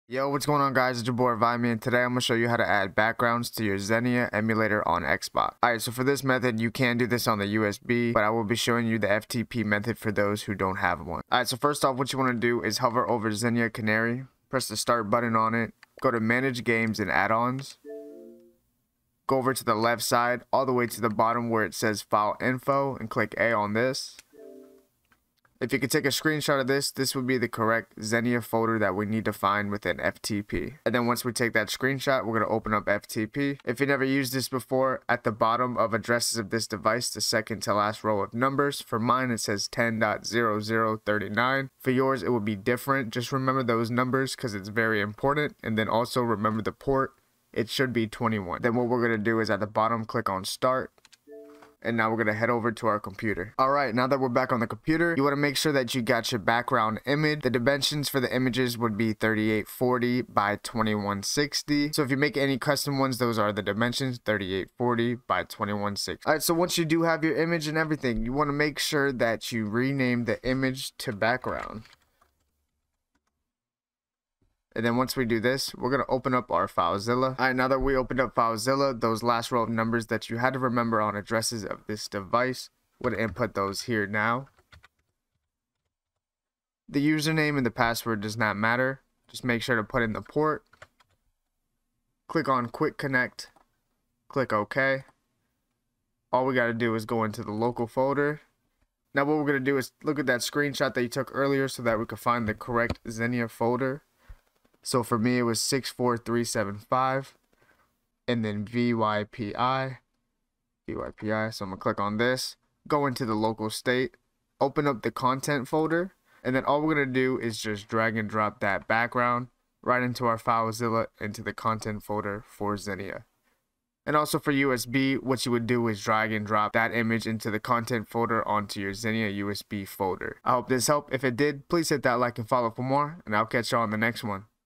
HOW TO ADD CUSTOM BACKGROUND TO XENIA ON XBOX !
In this video i will be showing how to add custom backgrounds to your Xenia app on xbox ! i will be showing FTP method :)
you can also do this with your USB just make sure to add the background into the contents folder inside your "Xenia USB Folder"